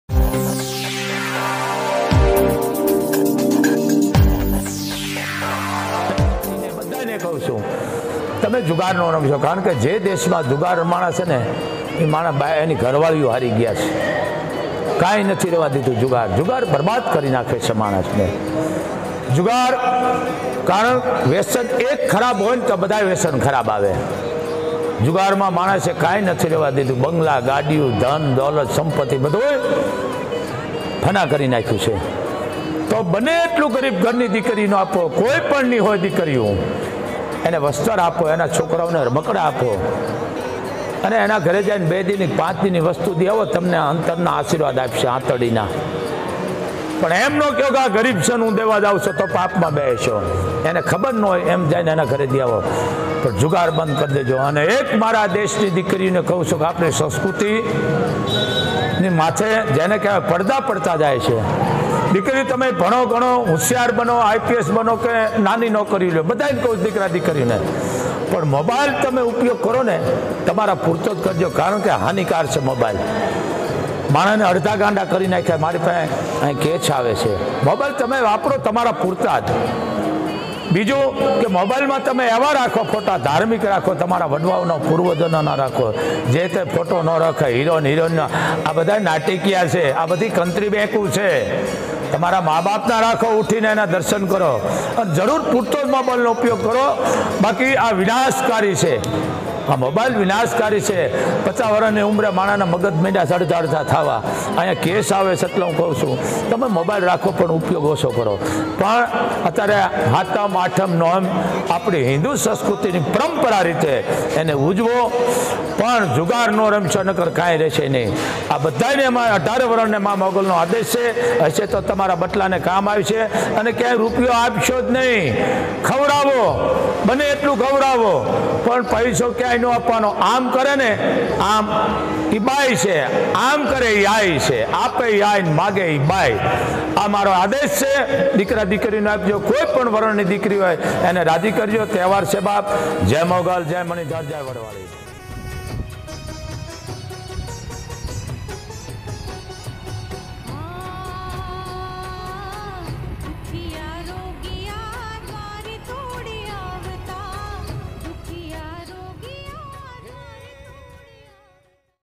મોગલ ધામ કબરાઉ બાપુ શ્રી જુગાર રમવા બાબતે જુગાર ના રમસો ગરીબ પરિવાર ને ખવરાવજો એને વસ્ત્ર આપજો ...
મોગલ ધામ કબરાઉ
જય માઁ મોગલ
જય મણિધર
જય વડવાળી
 બાપુ શ્રી એ જણાવ્યું જુગાર ના રમો અને ગરીબ ઘરના પરીવાર ને ખવરવો એન દાન આપો વસ્ત્ર આપો પણ રૂપિયા ક્યાંય નય આપશો આ મોગલ નો આદેશ છેઃ